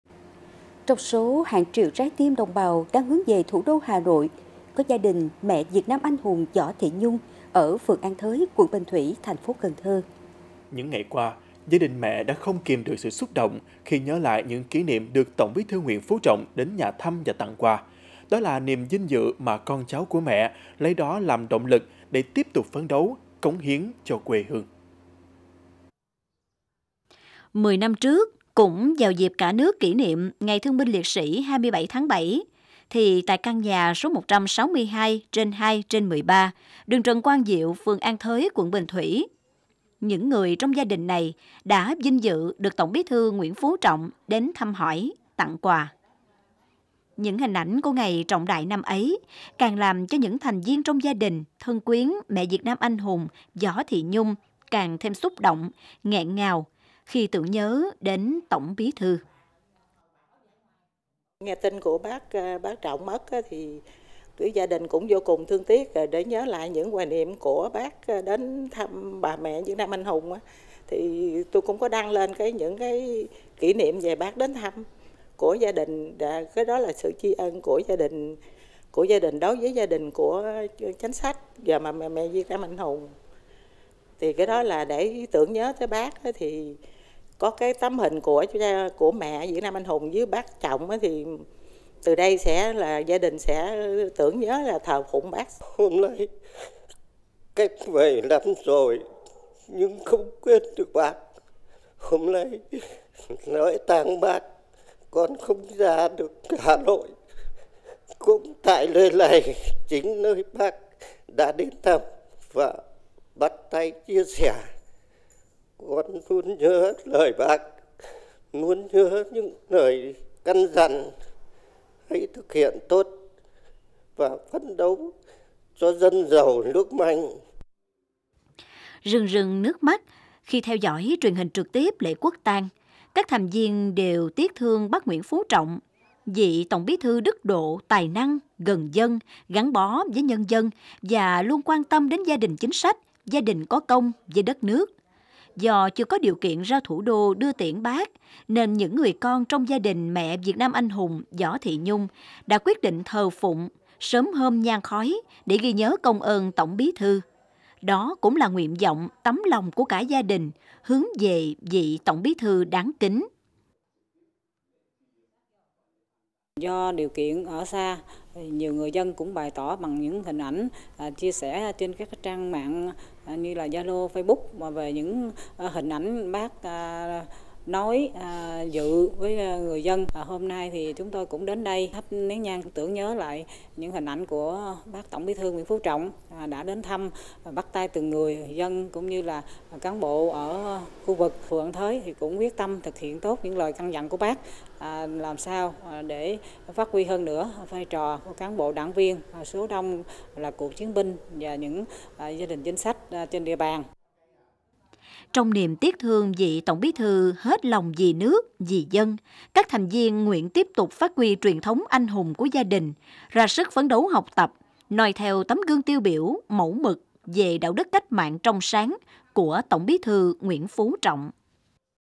Xúc động tiễn biệt Tổng Bí thư Nguyễn Phú Trọng | Cần Thơ TV
Những ngày qua, gia đình mẹ Việt Nam Anh hùng Võ Thị Nhung ở phường An Thới, quận Bình Thủy, TP. Cần Thơ đã không kìm được sự xúc động khi nhớ lại những kỷ niệm được Tổng Bí thư Nguyễn Phú Trọng đến nhà thăm và tặng quà, đó là niềm vinh dự mà con cháu của mẹ lấy đó làm động lực để tiếp tục phấn đấu, cống hiến cho quê hương.